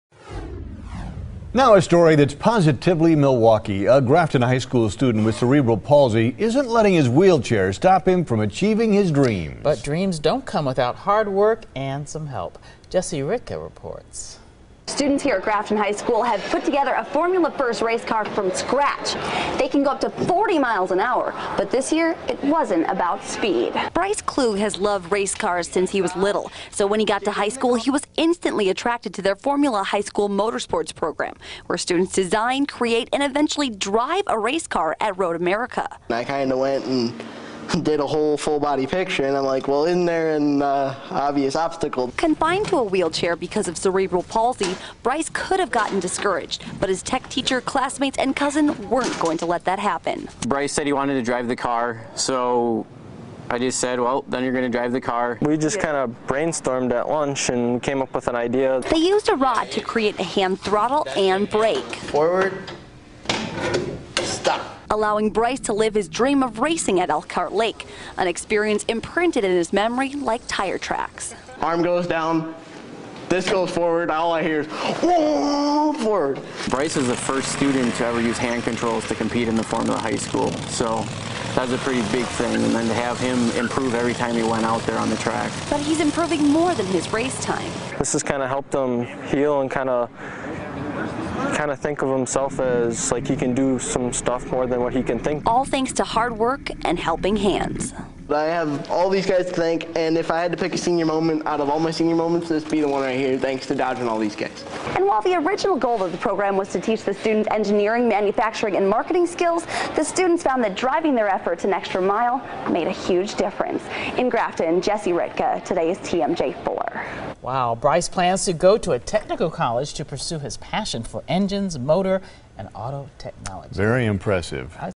Students design race car for boy with cerebral palsy
Students at Grafton High School put together a formula first race car from scratch each year.  The cars can go up to 40 miles per hour but this year, it wasn't just about speed.